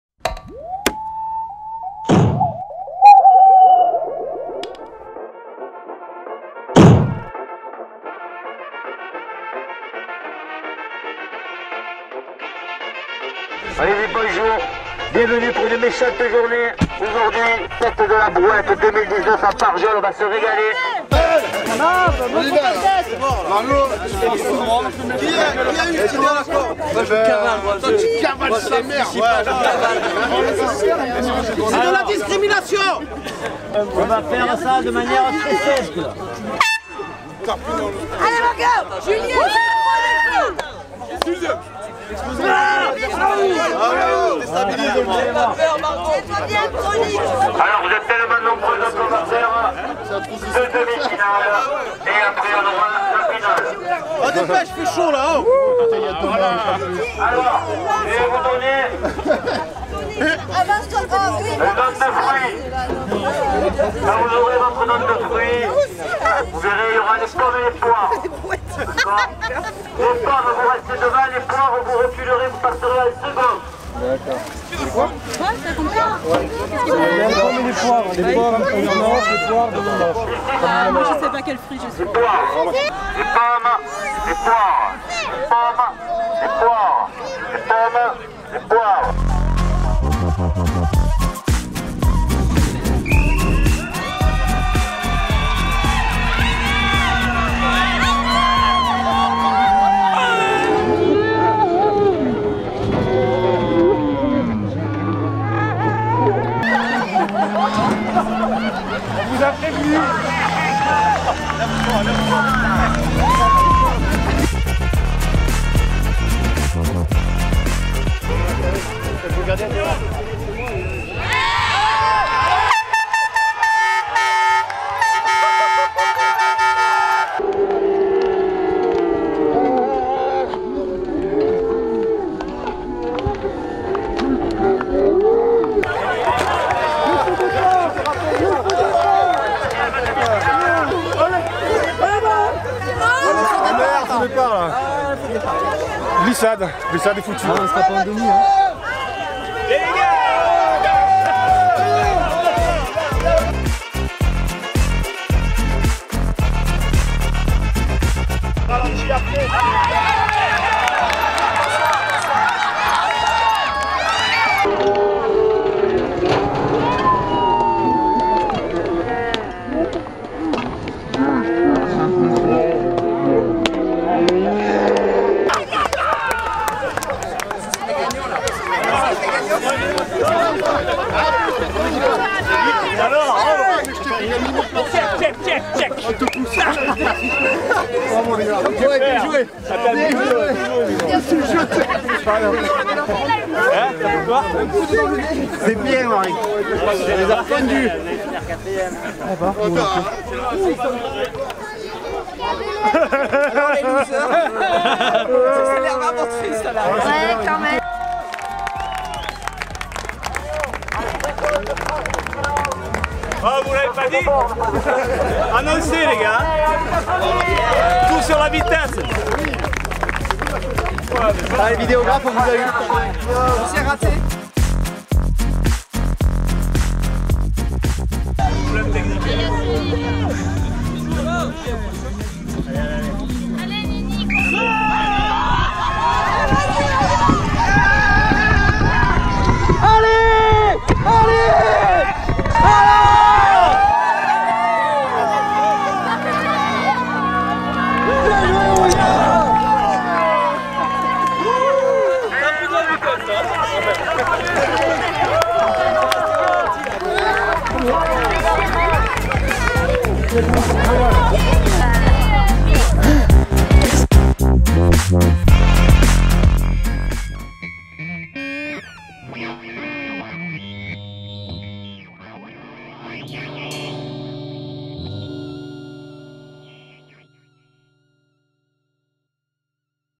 LA PELOTE #12 / Le sprint / Faites de la Brouette
L’épreuve du sprint lors de la "Faites de la Brouette" organisée par le Vide à Remplir à Barjols le 1er Mai 2019. Que des gagnants !